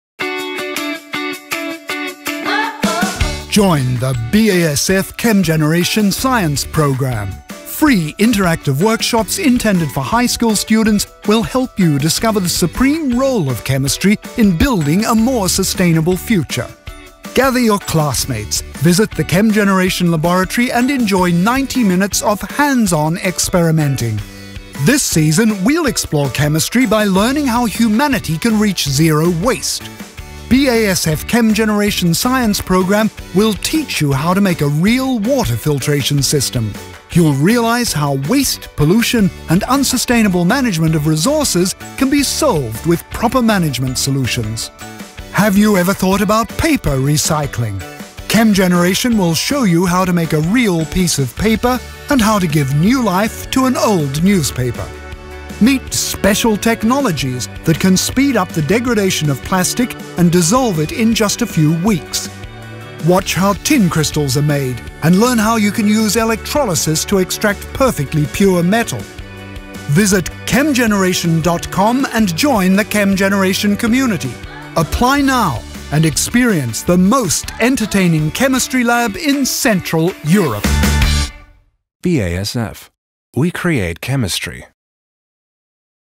BASF Chemgeneration Romania
Programul BASF Chemgeneration Science este un atelier de științe interactiv, a cărui obiectiv este acela de a populariza științele, de a-i implica pe elevii de liceu (cu vârste cuprinse între 14-18 ani) în descoperirea aspectelor fascinante ale chimiei și de a-i ajuta să înțeleagă modul în care științele naturii pot susține o dezvoltare durabilă pe Pământ.

Prin intermediul acestui program, elevii au ocazia de a participa la experimente interactive, de a vedea modul în care știința poate oferi soluții la probleme cotidiene și de a înțelege rolul ei în salvarea planetei. Elevii de liceu vor putea să vadă modul în care funcționează chimia în afara orelor de clasă, ceea ce le va stimula creativitatea și talentul în sfera științifică.

La programul Chemgeneration pot participa toți elevii, nu doar aceia care au rezultate bune la chimie sau care manifestă un interes aparte față de științele naturii. Programul le este adresat liceelor care oferă educație generală, nu doar acelora care au secții specializate de științe naturale.

Programul Chemgeneration Science este oferit de BASF, liderul mondial în produse chimice, în colaborare cu unele dintre cele mai prestigioase universități și instituții științifice din Europa centrală.